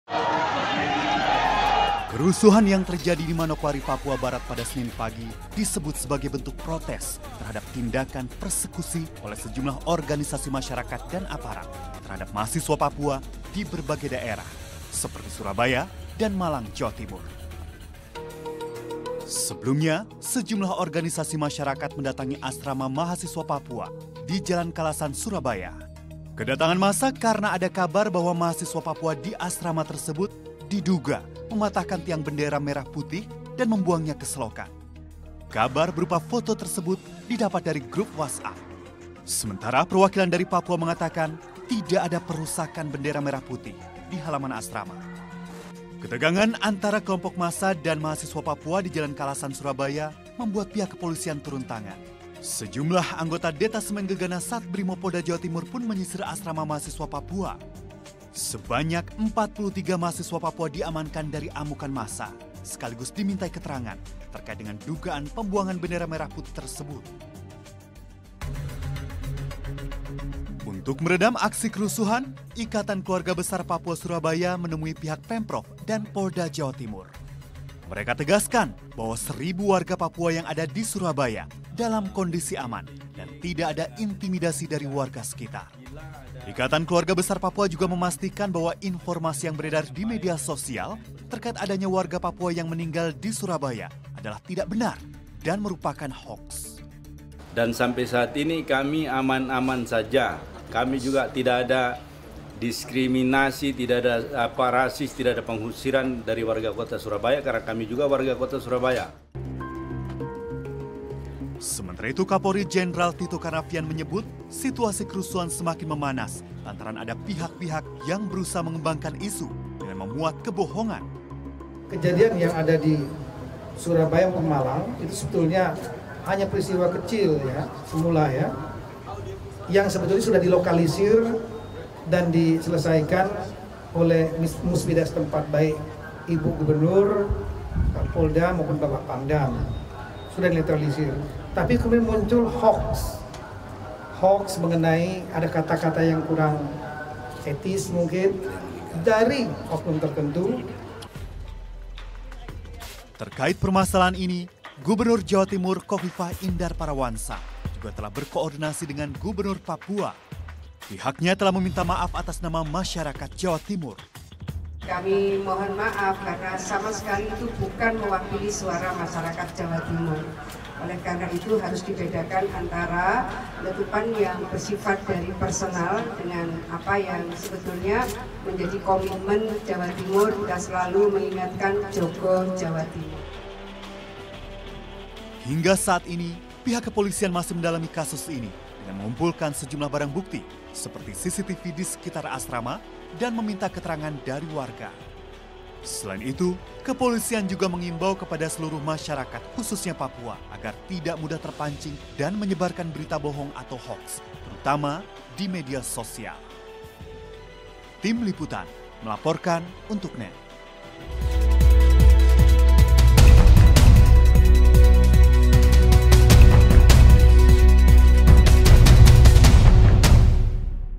Kronologi Kerusuhan Manokwari - NET YOGYA
(19/08) Kerusuhan yang terjadi di Manokwari dan sejumlah daerah lain di Papua mulai mereda pada senin malam. Tak dipungkiri unjuk rasa tersebut berawal dari insiden Asrama Mahasiswa Papua di Surabaya dan Malang.

Kerusuhan yang terjadi di Manokwari, Papua Barat pada Senin Pagi tersebut sebagai bentuk protes terhadap tindakan Persekusi dan Rasisme oleh sejumlah Organisasi Masyarakat dan Aparat, terhadap Mahasiswa Papua di berbagai daerah seperti Surabaya dan Malang.

57 UHF Pkl. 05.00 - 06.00